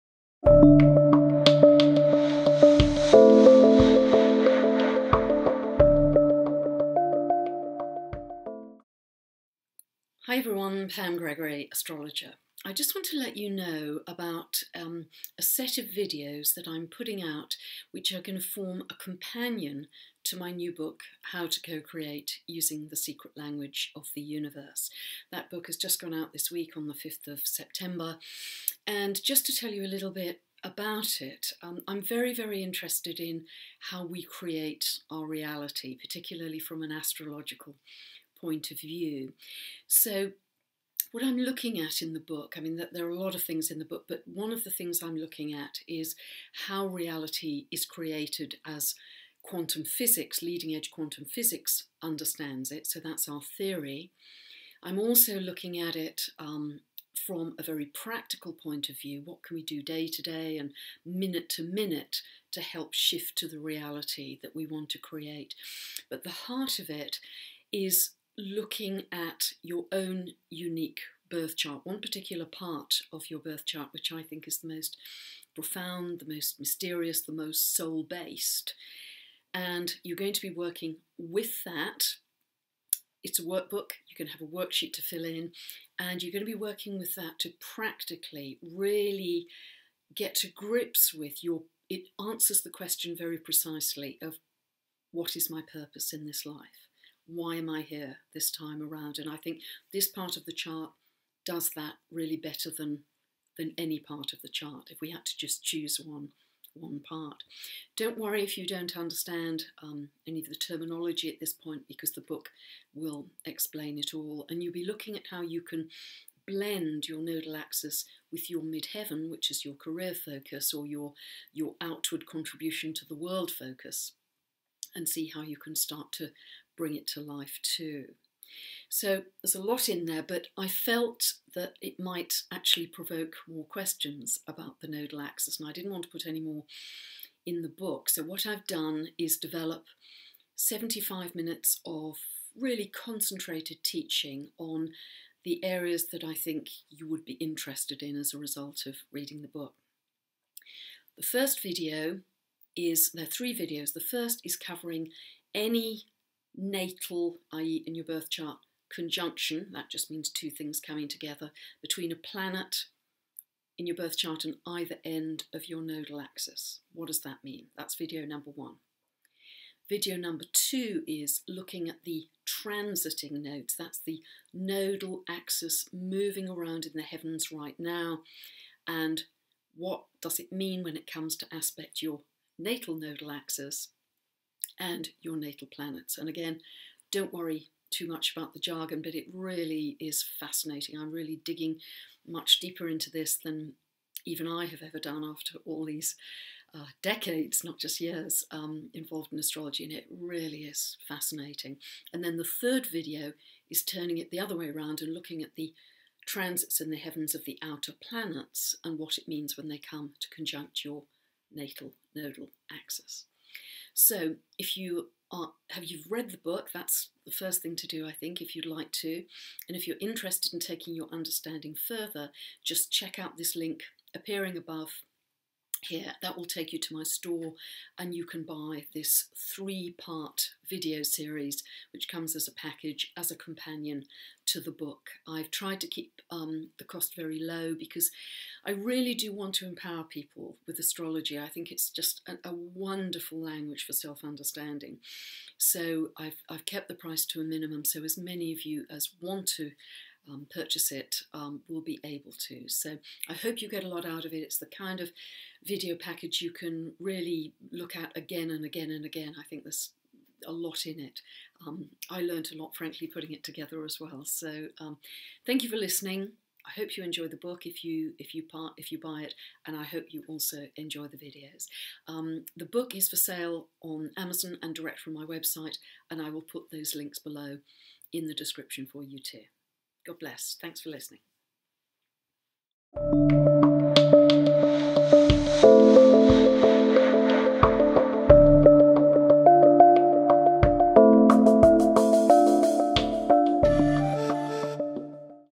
Video Companion to How to Co Create using the Secret Language of the Universe
Pam talks about the 3 part Video Companion to her latest book How to Co-Create using the Secret Language of the Universe. If you would like to purchase this 3 part series, just click on this If you would like to buy the book, you can do this from Amazon.com or Amazon.co.uk. Enjoy!